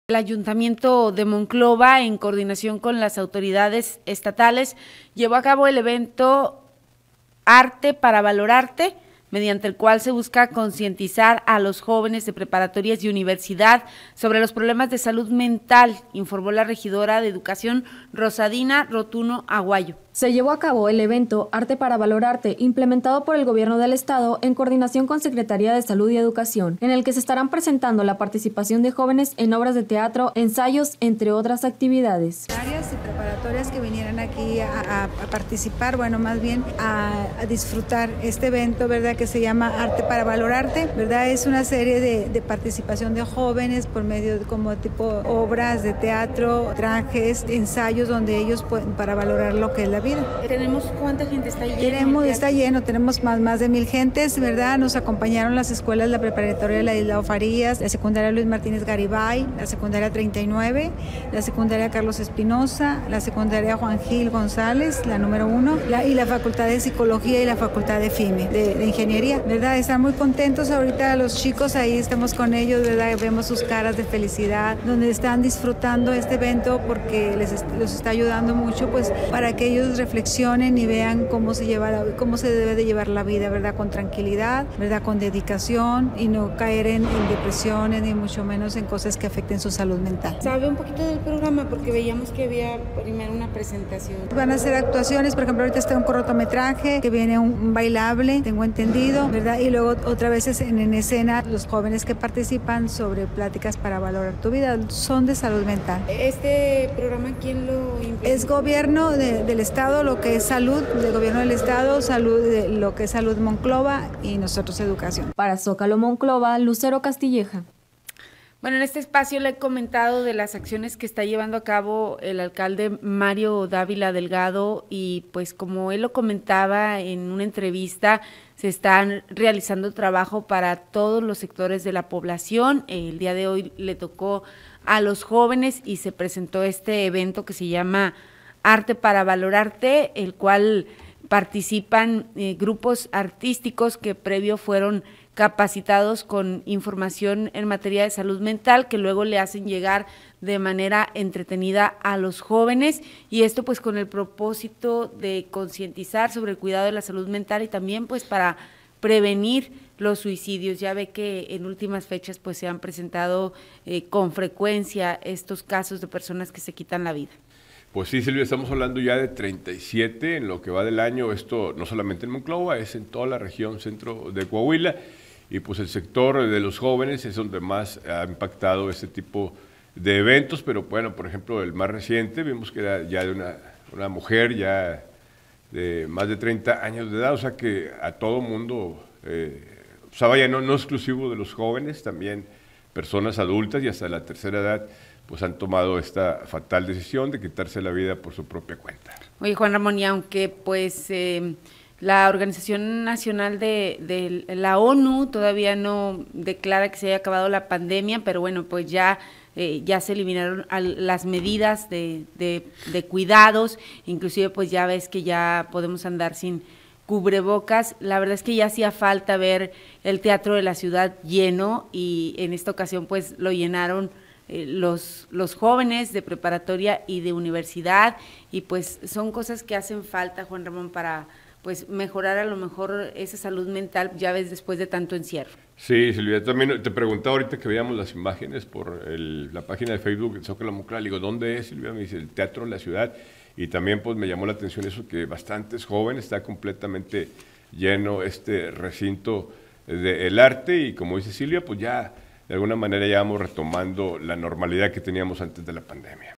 Se llevo a cabo el evento de Arte Para Valorarte
El Ayuntamiento de Monclova en coordinación con autoridades estatales llevo a cabo el evento de Arte Para Valorarte, mediante el cual se busca concientizar a los jóvenes de preparatorias y universidades, sobre los problemas de salud mental, informó la regidora de educación, Rosadina Rotuno Aguayo